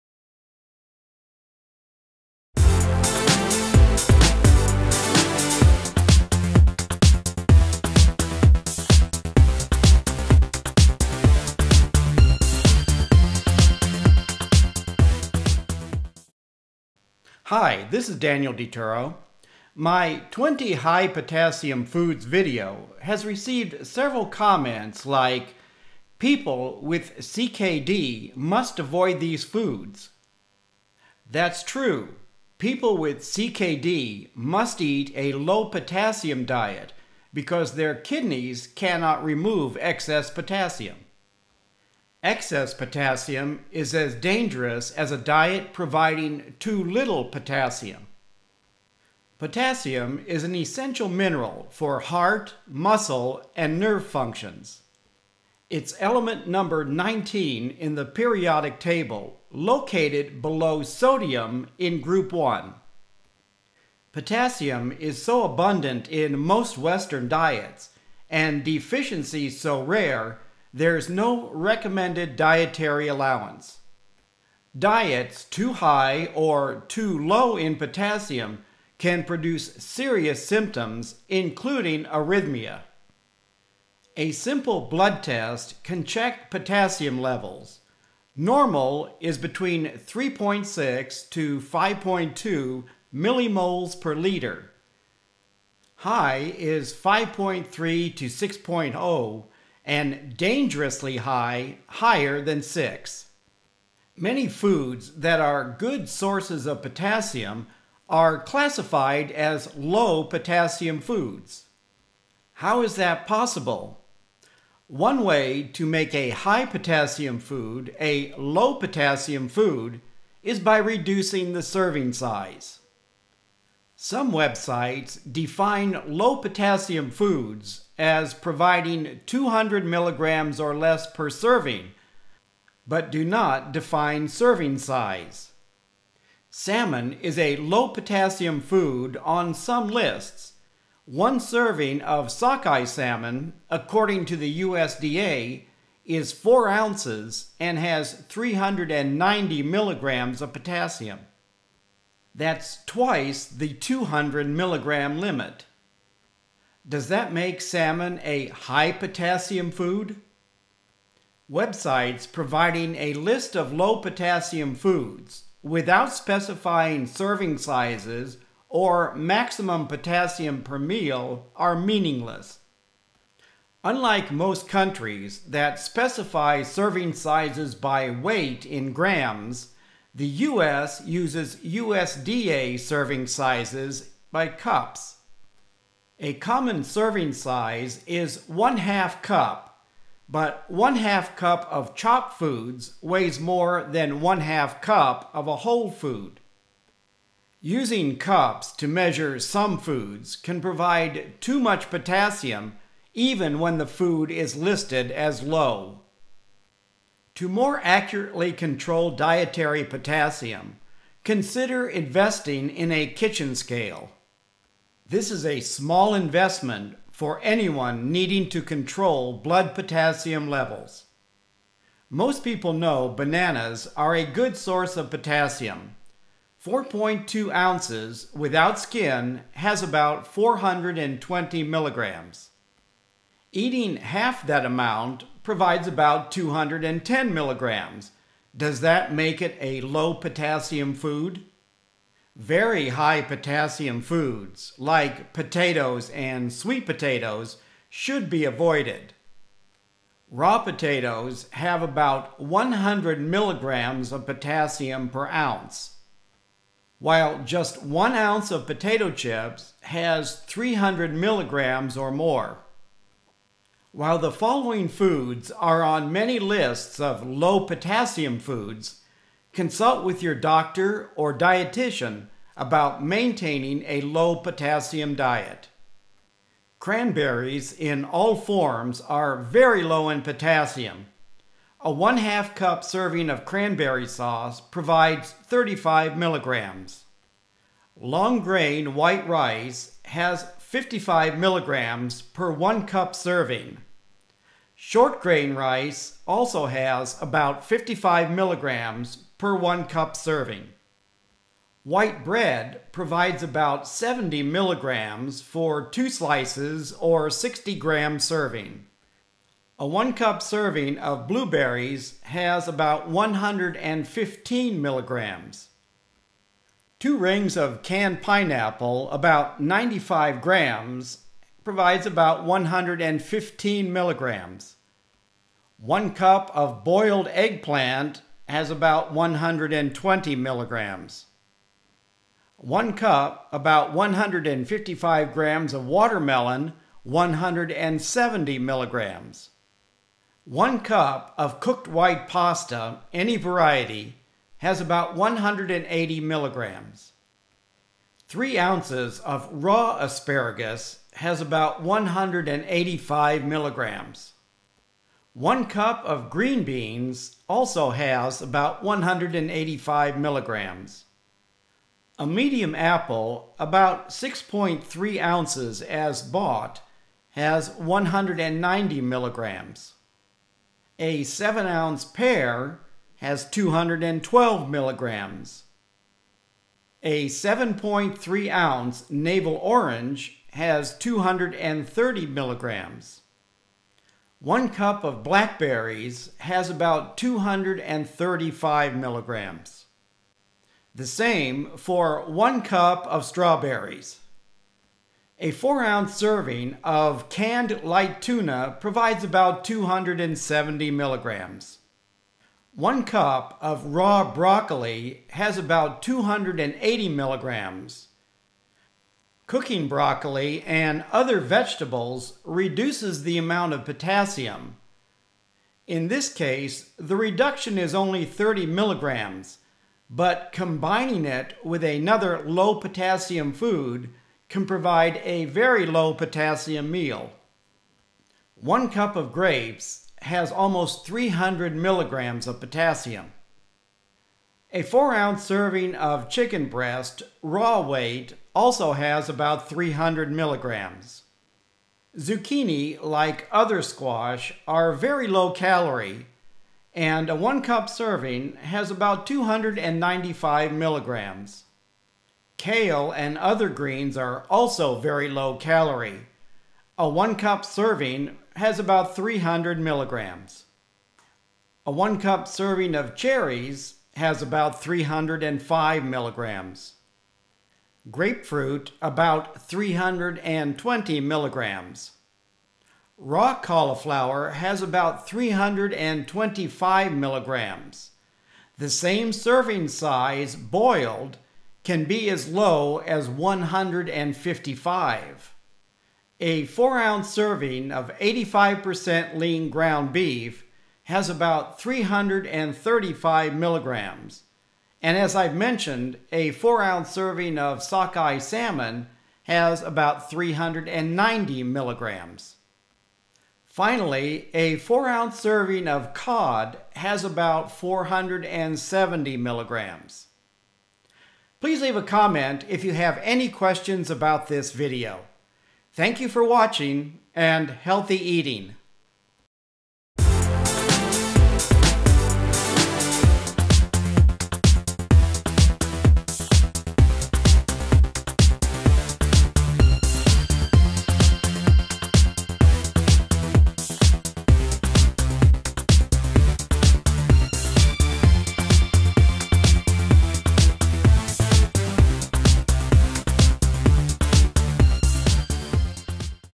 30 Low Potassium Foods (700 Calorie Meals) DiTuro Productions
30 foods considered low potassium for people with chronic kidney disease or other medical condition requiring a low potassium diet.